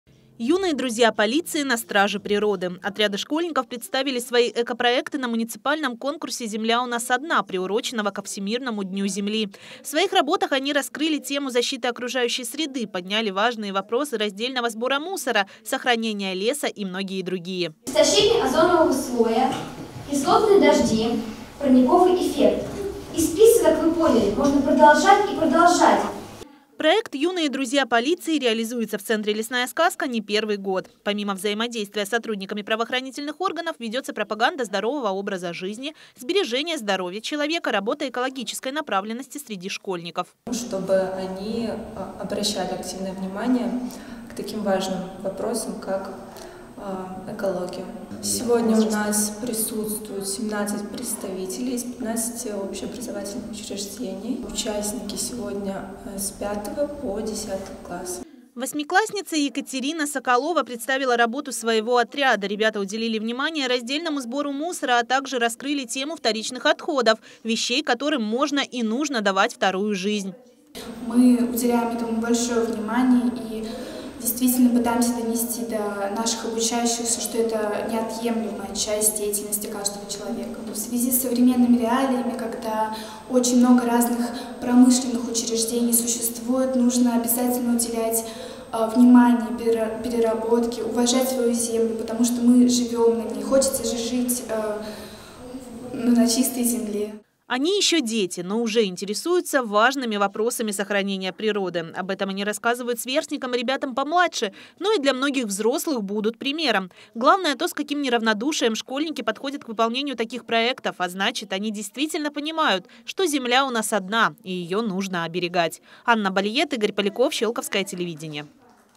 Юные друзья полиции представили проекты на конкурсе «Земля у нас одна»
Отряды школьников «Юные друзья полиции» представили свои экопроекты на муниципальном конкурсе «Земля у нас одна», приуроченном ко Всемирному дню Земли. Мероприятие состоялось в центре «Лесная сказка».